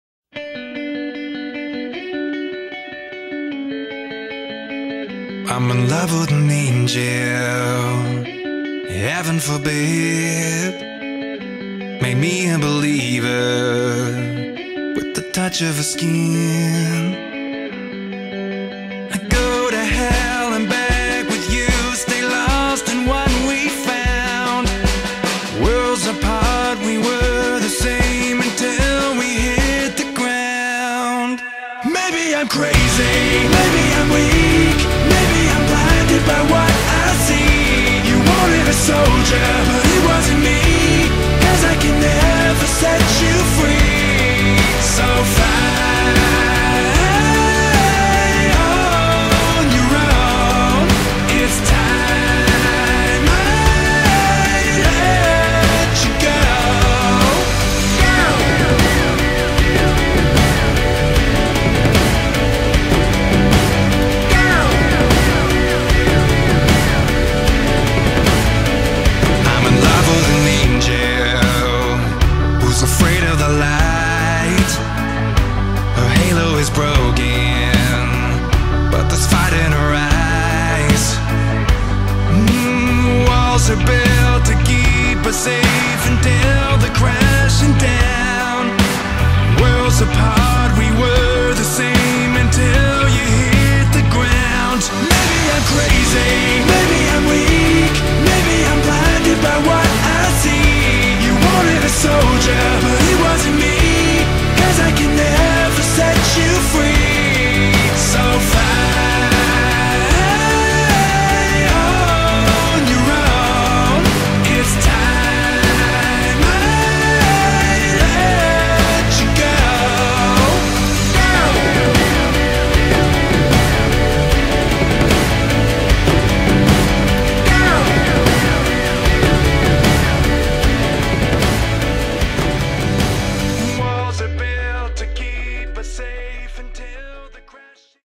Trail Running por el Embalse de Malpasillo ( Badolatosa )
Tarde de Trail, por los senderos de Corcoya y el Embalse de Malpasillo ( Badolatosa )